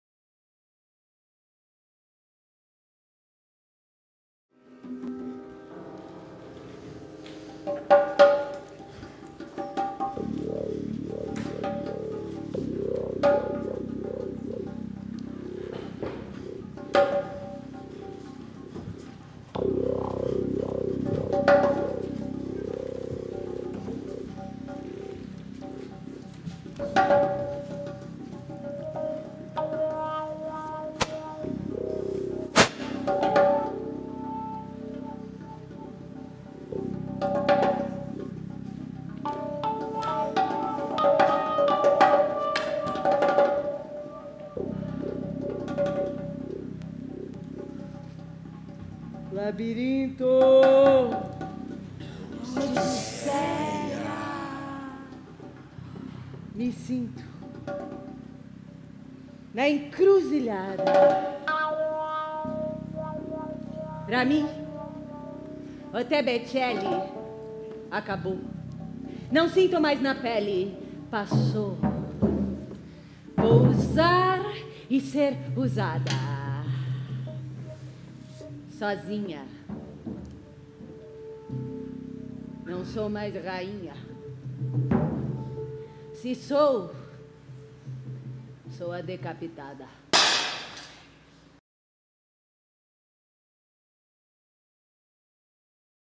Antígone - Cacilda!!!!! - A Rainha Decapitada - Teatro Oficina
Sylvia Prado interpreta Cacilda Becker / Antígone

Cacilda 5 é escrita por José Celso Martinez Correa e Marcelo Drummond
Direção José Celso Martinez Correa

imagens: Igor Marotti